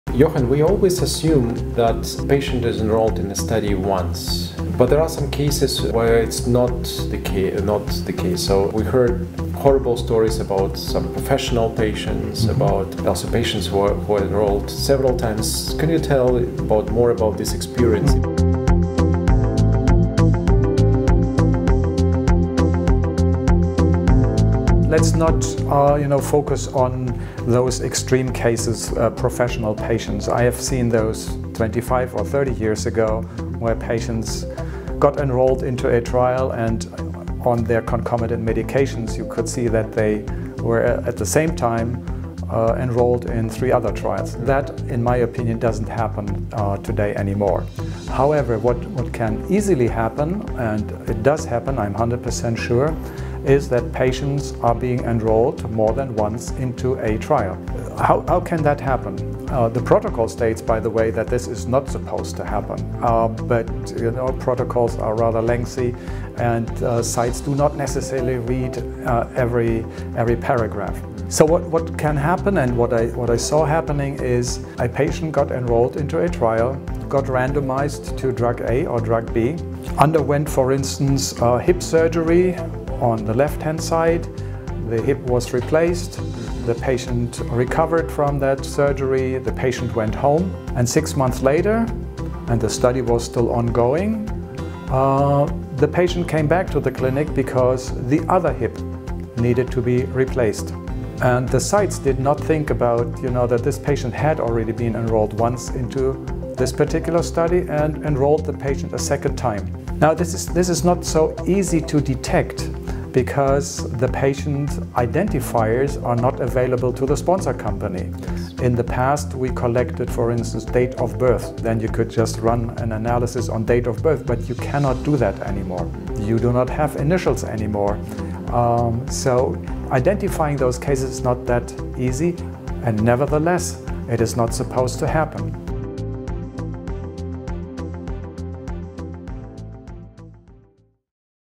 Can one patient be enrolled in a study more than once? | MyRBQM Experts
Duplicate subjects is something we all have on our plate. Centralized Statistical Monitoring (CSM) can drill down to 'duplicates' even on a record level within a patient or record level across patients. Learn all about what CSM can do for you with, among others, the use of digit preference algorithms